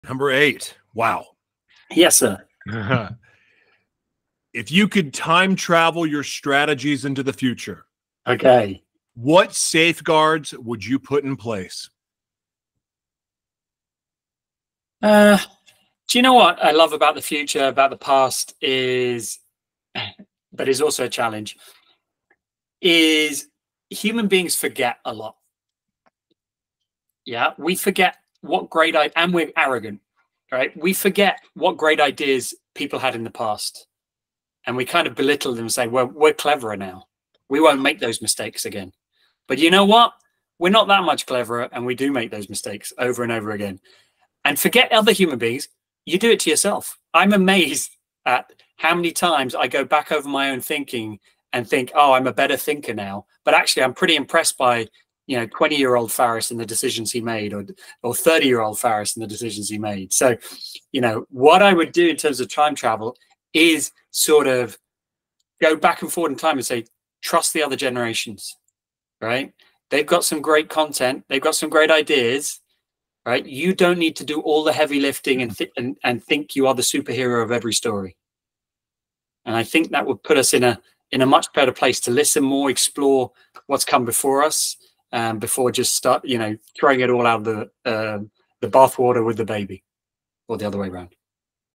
Time-Traveling Strategies: Balancing Future Innovation with Timeless Foundations
In this thought-provoking segment of "UnMasking Humanity," Faris Aranki - Strategy and Emotional Intelligence shares a remarkable story of how focusing on basic meeting efficiency dramatically improved a company's sales performance.

Joshua T Berglan then expands on the importance of blending futuristic thinking with time-tested principles.

This discussion highlights the critical balance between embracing cutting-edge technologies and preserving foundational business practices, emphasizing the value of intergenerational wisdom in shaping successful strategies for the future.

Key Points:

Unexpected Solutions: Aranki's counterintuitive approach of improving meeting efficiency to boost sales demonstrates the power of addressing fundamental organizational issues.

Small Changes, Big Impact: The anecdote illustrates how seemingly minor adjustments can lead to significant improvements in company performance.

Futurism vs. Foundational Knowledge: Berglan stresses the importance of understanding both future trends and historical business principles.

Intergenerational Learning: The discussion emphasizes the value of knowledge exchange between different age groups in business and technology.

Balancing Innovation and Tradition: The segment highlights the need to integrate new technologies with established business practices for optimal results.

Wisdom of Elders: Berglan advocates for respecting and learning from older generations' experiences and insights.

Holistic Approach to Progress: The conversation underscores the importance of a well-rounded approach to business strategy that combines past lessons with future innovations.

Call to Action:

Dive deeper into this fascinating discussion on balancing future-oriented strategies with timeless business principles.